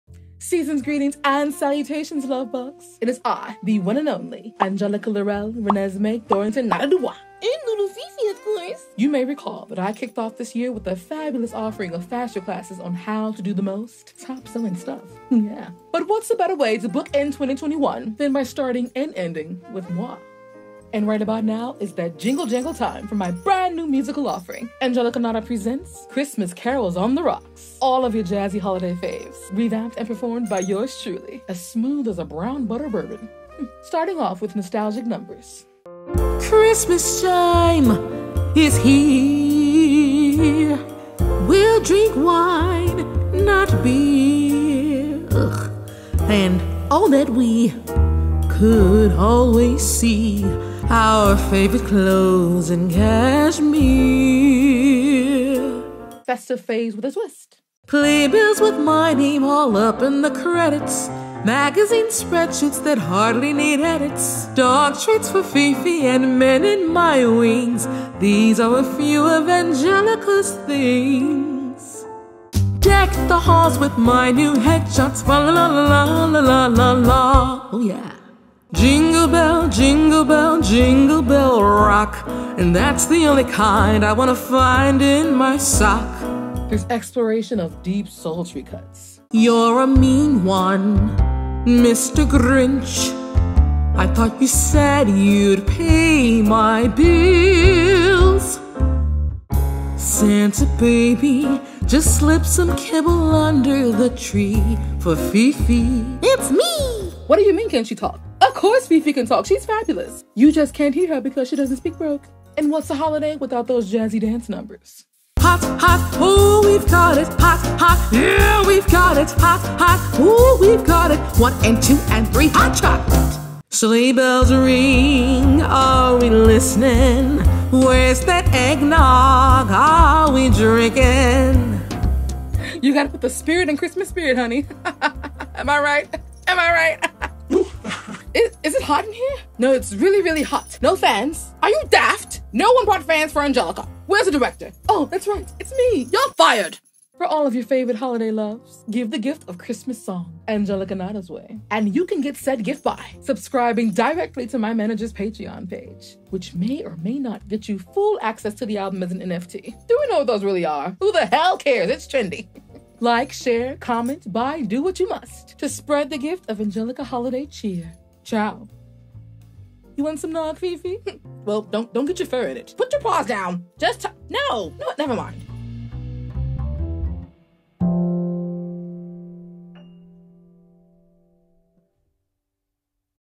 Angelica Nada Presents: Christmas Carols on the Rocks
Angelica Nada DuBois is BACK for the holidays, darlings! She & FiFi return with a seasonal offering of self-produced Christmas Carols fit for the bourgeoisie. Enjoy your favorite festive tunes with a glass of egg nog fit for the occasion.

Filmed, scripted & edited by B~Free

Studio scene holiday music arranged, performed & recorded by B~Free